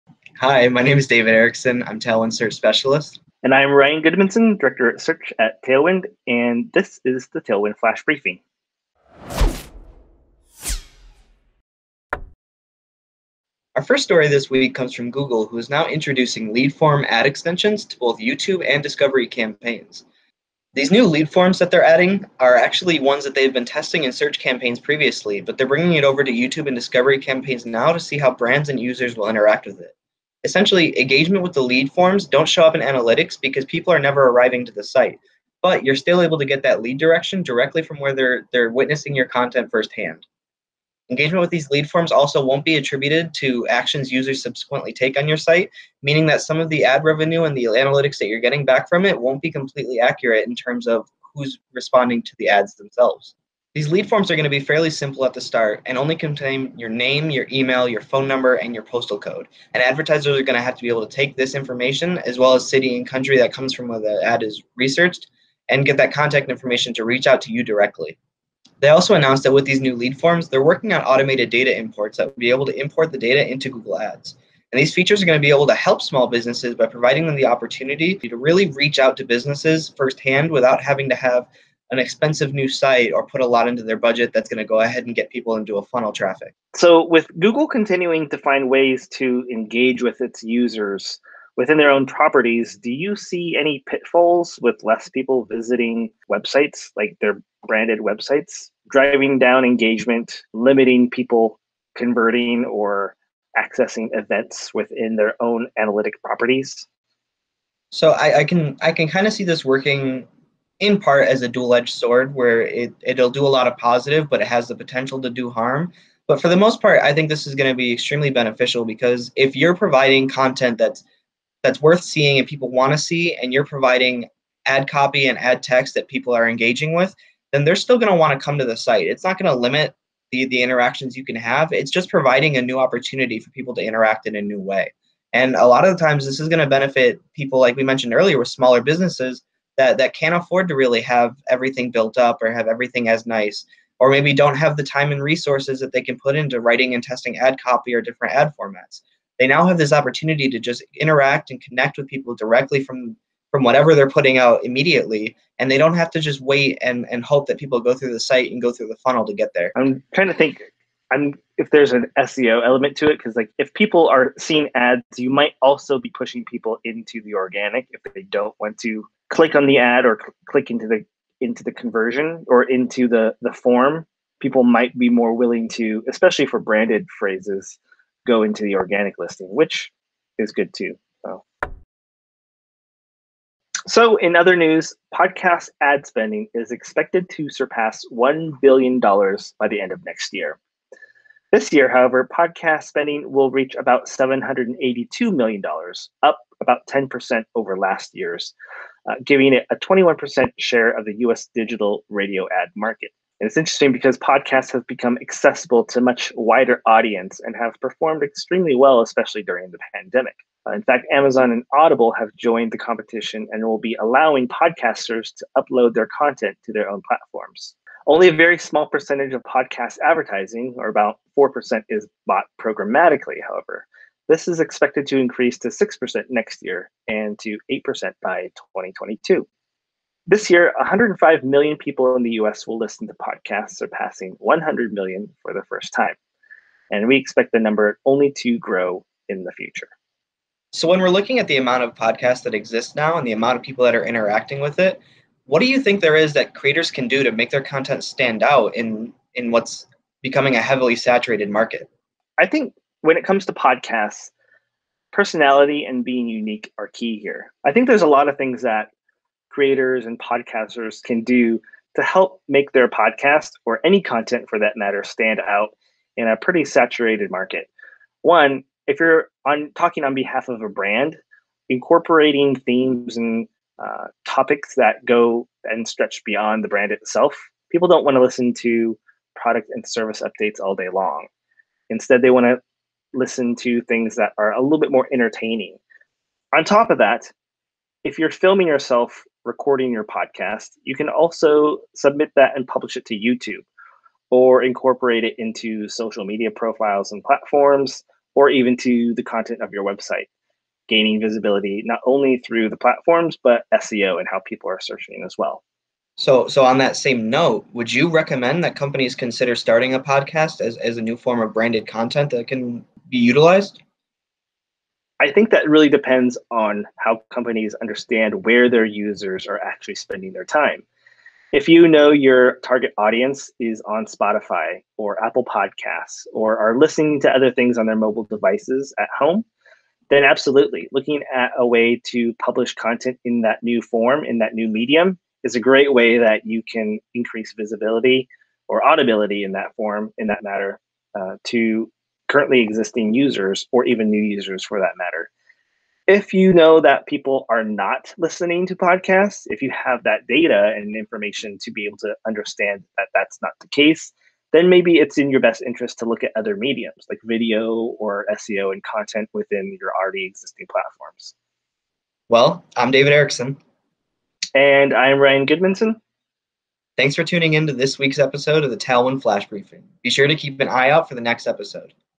Google Lead Form Extensions & Podcast Ad Spending | Flash Briefing Week of 8/10
In this week's briefing, Search Specialist, Dave Ericson & Director of Search, Ryan Gudmundson discuss Google's introduction of lead form extensions, and the rising amount of ad spending going towards podcasts. Will you experiment with Google lead forms? Does your company have your own podcast? Let us know!